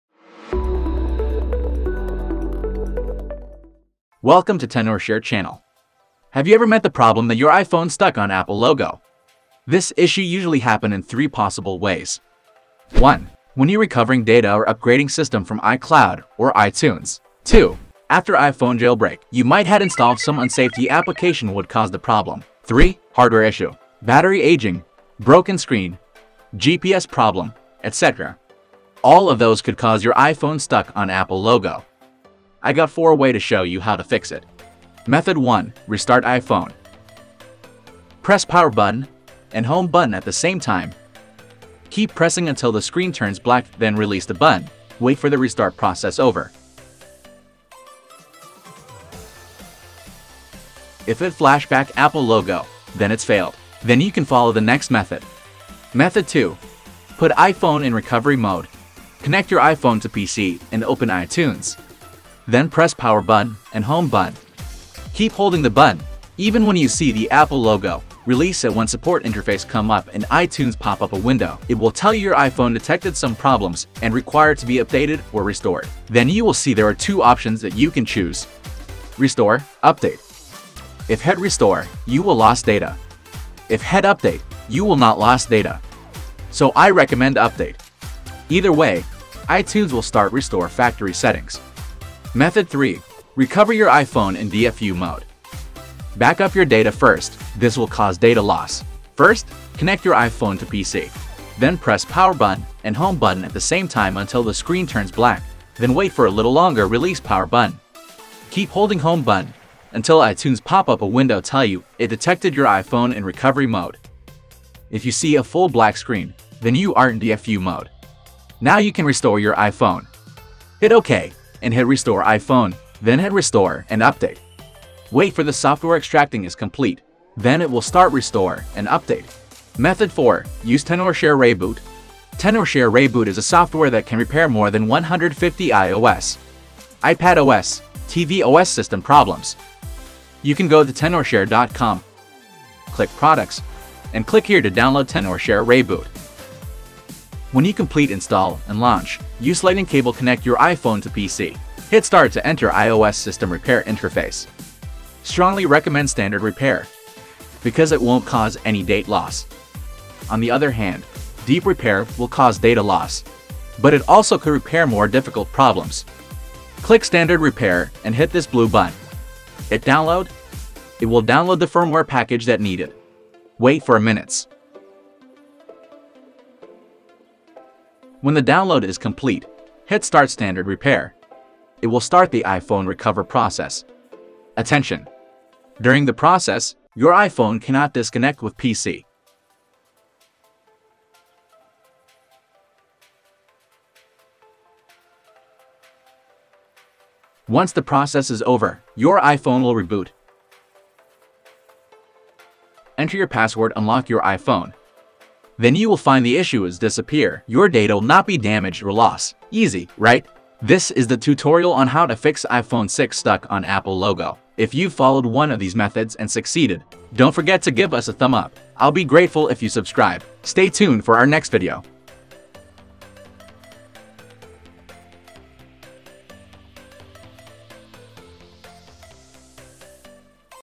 How to Fix iPhone 6s Stuck on Apple Logo 2021
Still upset about iPhone 6s stuck on Apple logo? Don't worry, this video wil tell you what cause the problem occur and show you 4 ways to fix it.

Timestamps:
00:00 Intro & Reasons Cause The Issue
00:35 Restart iPhone
00:56 Repair In Recovery Mode
01:33 Repair In DFU Mode
02:10 Repair with ReiBoot